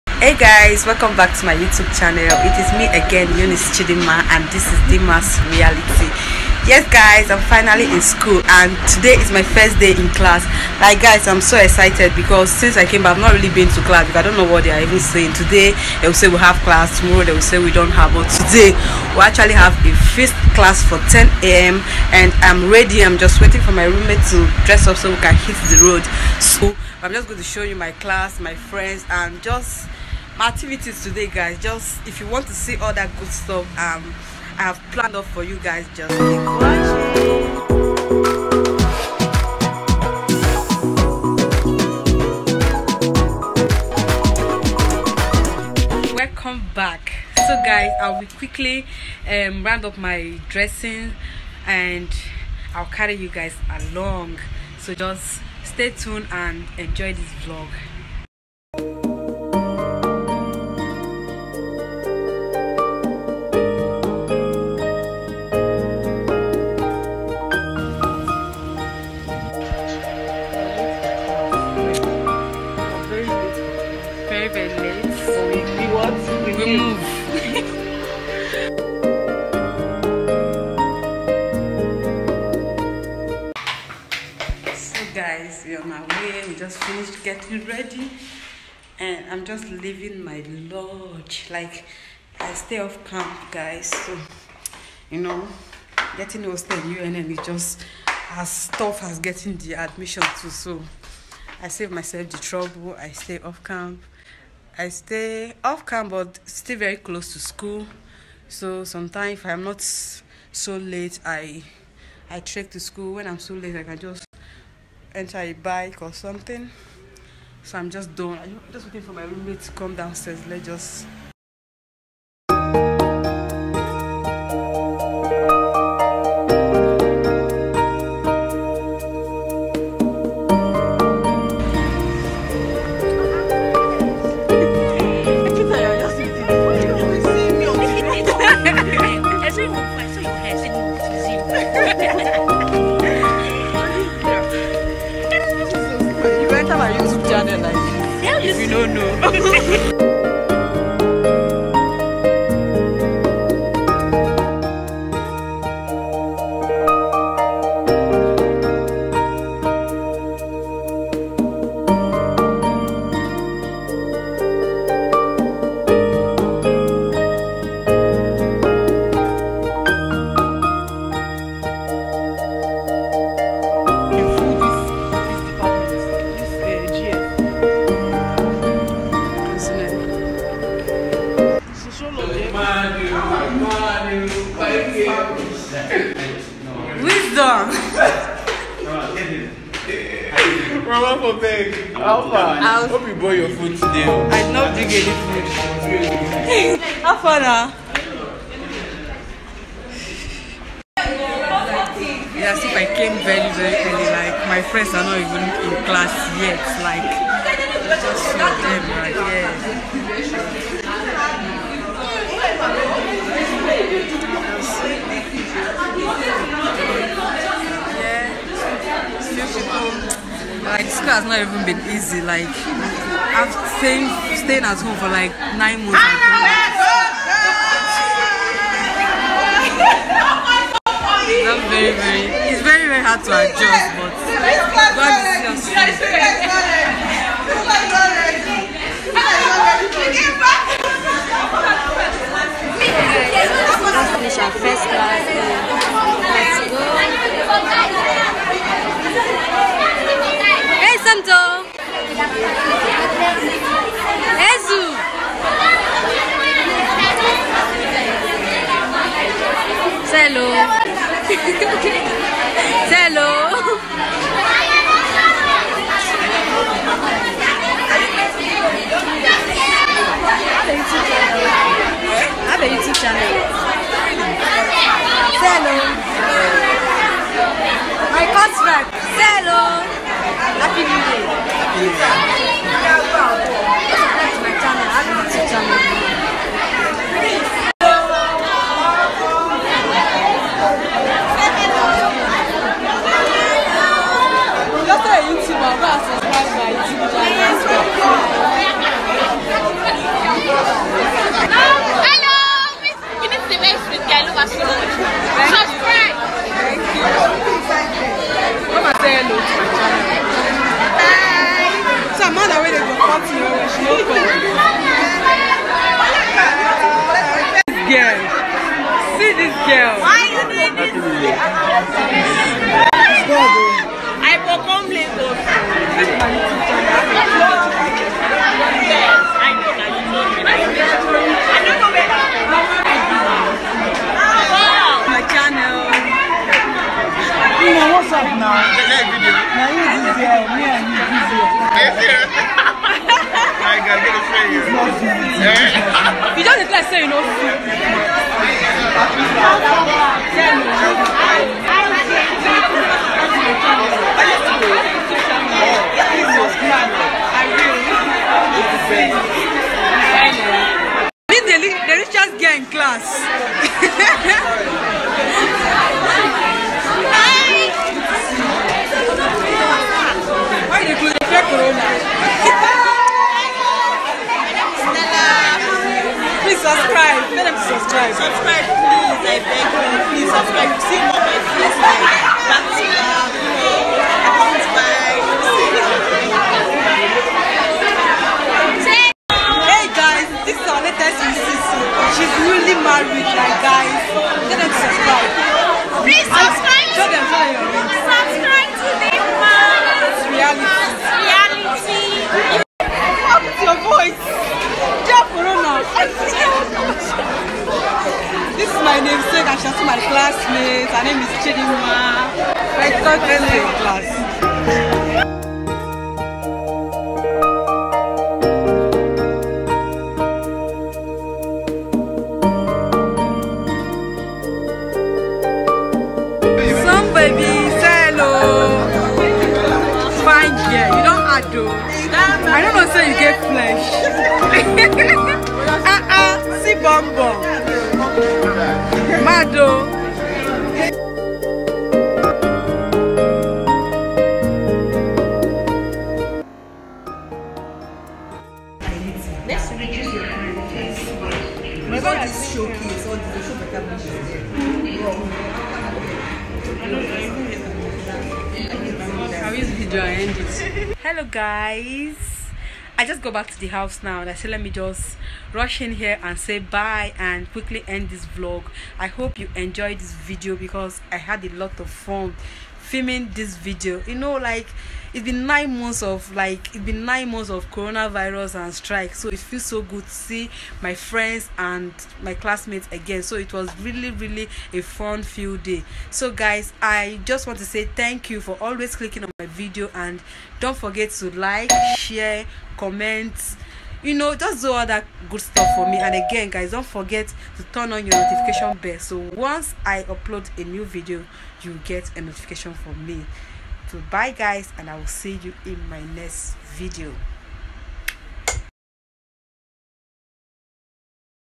First day at school🥰🥰🥰 dimma’s reality# school vlog#
Hello beautiful people, so on today’s video I carried you guys along with me to school, as I went  to my first class today after 9 months🤗🤗 it was fun and I hope you enjoy this video.